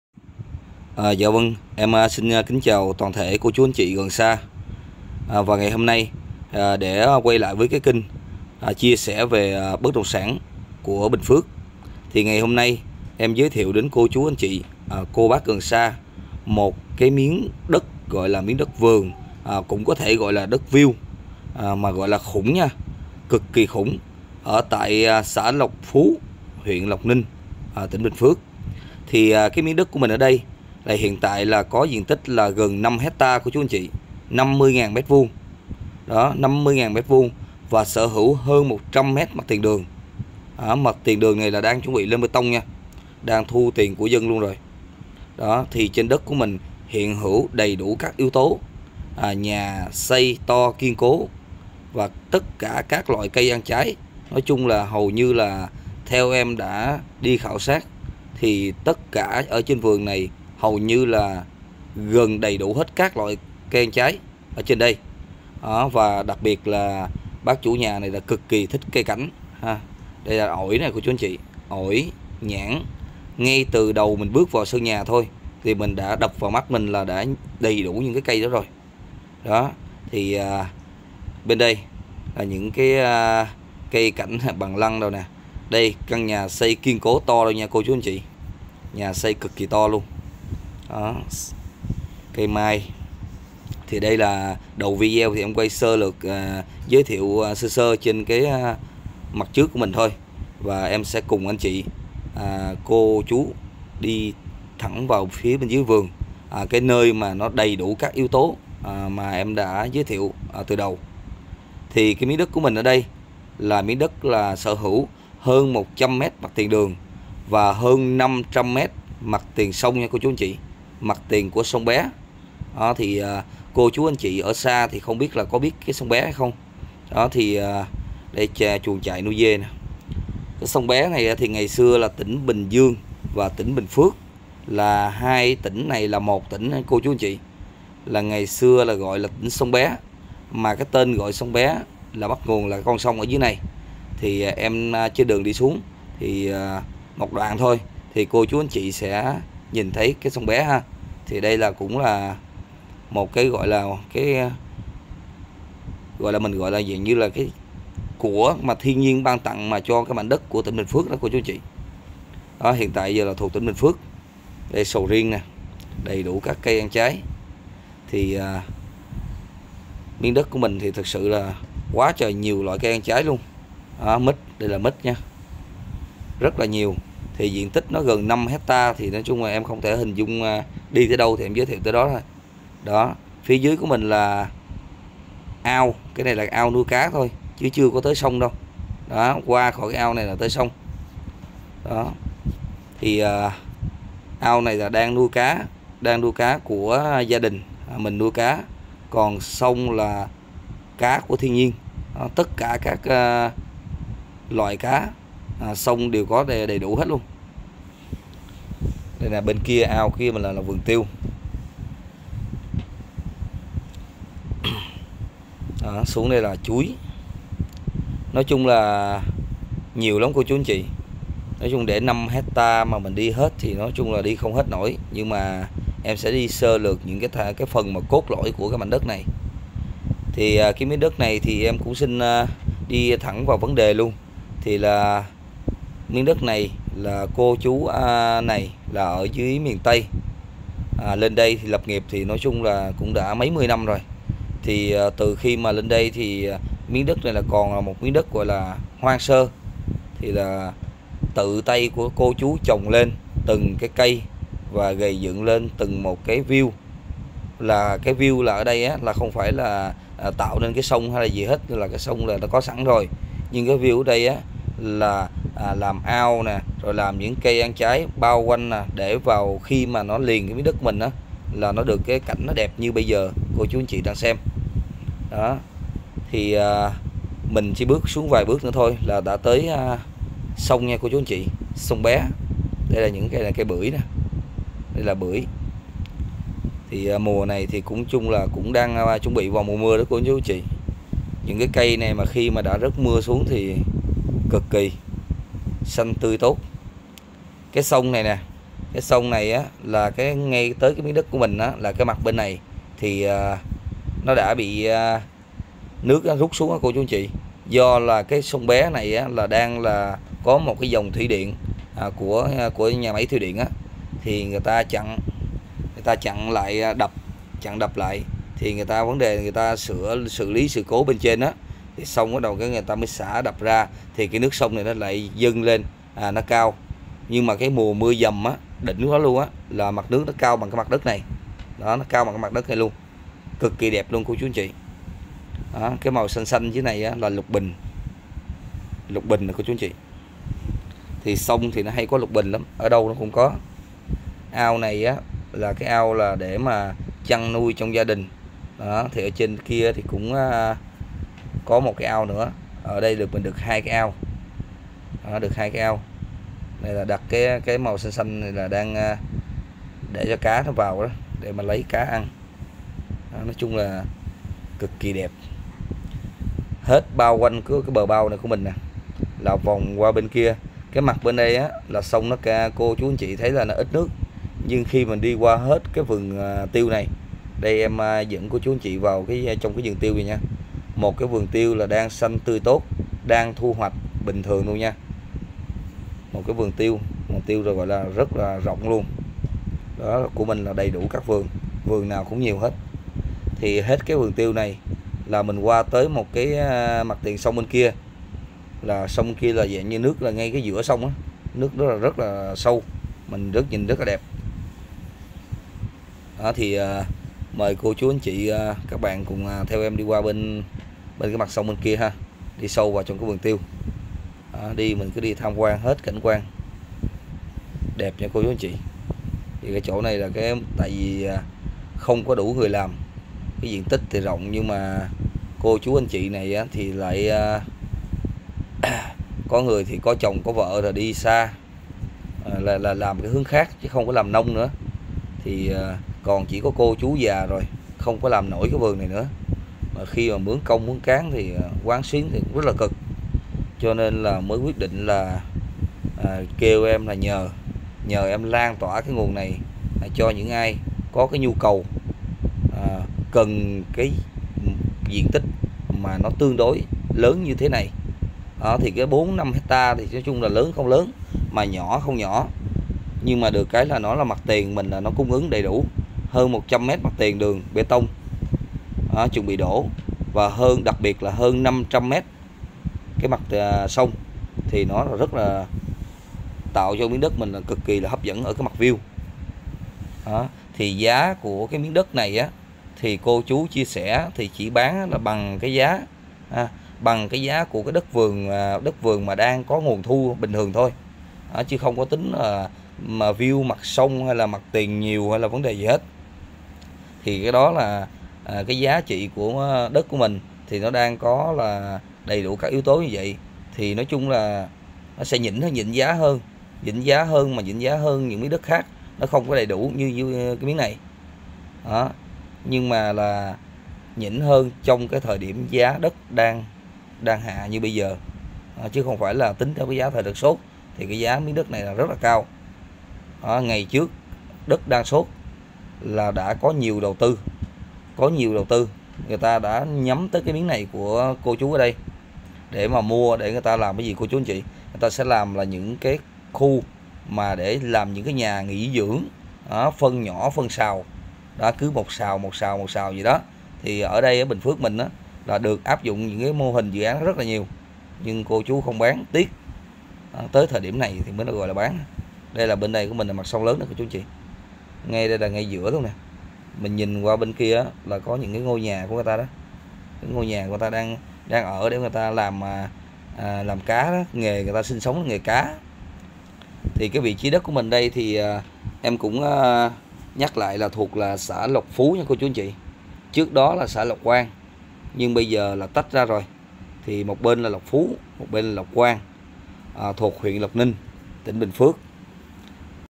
Đất Nhà Vườn & Viu Sông Bé Siêu Khủng Tại Lộc Phú-Lộc Ninh-Bình Phước. Cần Tìm Chủ Mới. Giá Hợp Lý
Đất Nhà Vườn & Viu Sông Bé Siêu Khủng Tại Lộc Phú - Lộc Ninh - Bình Phước.
🔥Cần Tìm Chủ Mới. Giá vô cùng Hợp Lý

 🔴 Tdt: 4,5 héc ta. có hơn 100m mặt tiền đường đất chuẩn bị lên bê tông. hơn 500m mặt Viu sông bé. điện vào tới nơi. dân cư hiện hữu. ( sổ sẵn cầm tay )

___Cách Chợ, Trường, Trạm..bán kính 3km
👉 Trên Đất có đầy đủ các loại cây trái cho thu nhập cao như Sầu Riêng, mít thái, bưởi, tiêu. Chuồng trại nuôi dê. ao to nuôi cá...
👉 Nhà xây to kiên cố... Vào là ở

💥 Đầu Tư hoặc Làm Của Đều Ok